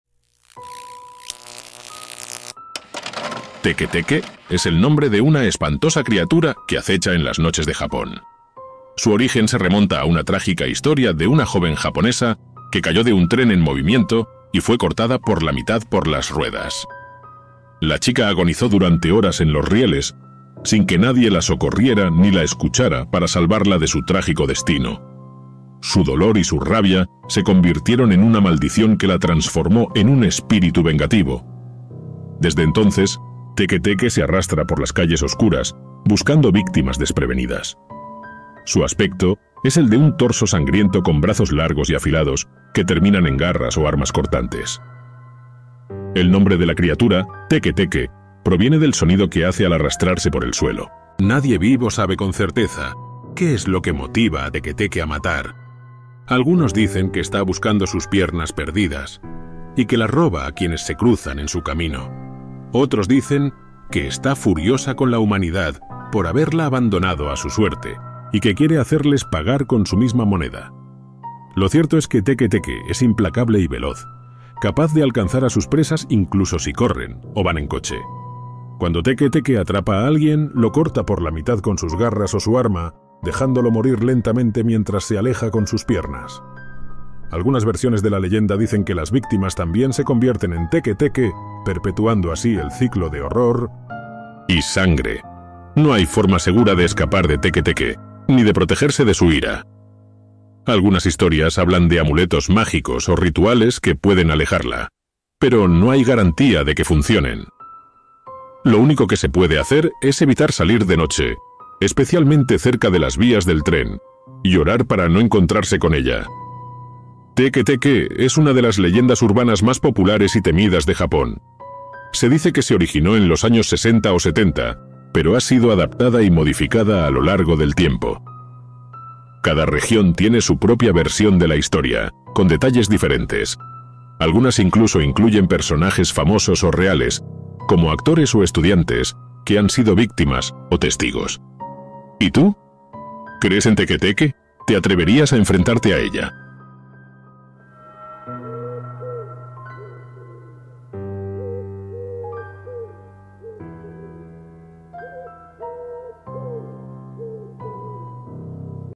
🚇 Esta noche en el metro...TEKE TEKE
Bajo el manto oscuro de la noche, donde los susurros de los árboles y el crujir de las hojas secas son tu única compañía, te sumergirás en una leyenda macabra que ha atemorizado a las mentes más osadas: la terrorífica historia del Teke Teke.

En un pequeñito pueblo japonés, vivía una joven llamada Aiko. Ella era una chica solitaria, atrapada en su propio mundo de desesperación y tristeza. Un día, acosada por un grupo de matones, Aiko huyó aterrorizada, buscando refugio en las oscuras vías del tren que cruzaban su pueblo.

Aquellos que han tenido la desgracia de cruzarse con el Teke Teke relatan encuentros aterradores. La criatura se abalanza sobre sus víctimas, persiguiéndolas sin piedad. Si te atrapa, no hay escapatoria.
💡 Soy_Curioso, tu canal de curiosidades y descubrimientos impactantes
💬 Deja un comentario para saber tus impresiones

Cuando el tren se acercaba, Aiko sintió un escalofrío paralizante y un miedo indescriptible. Justo en el momento en que las luces del tren la iluminaron, perdió el equilibrio y cayó sobre las vías. El rugido de las ruedas de acero sobre la tierra y los gritos de Aiko se mezclaron en una sinfonía de horror, y su cuerpo quedó partido en dos.

Desde aquel trágico día, el espíritu de Aiko vaga en busca de venganza. Se dice que en las noches de luna llena, cuando la oscuridad es más profunda y el viento susurra susurros inquietantes, el Teke Teke acecha a los incautos. Su aspecto es una pesadilla hecha realidad: la mitad inferior de su cuerpo ha desaparecido, dejando a la vista sus órganos internos y huesos sangrantes. A medida que arrastra su cuerpo mutilado, sus dedos desgarrados emiten un sonido siniestro: "teke, teke, teke".

¡Esperamos que te haya gustado!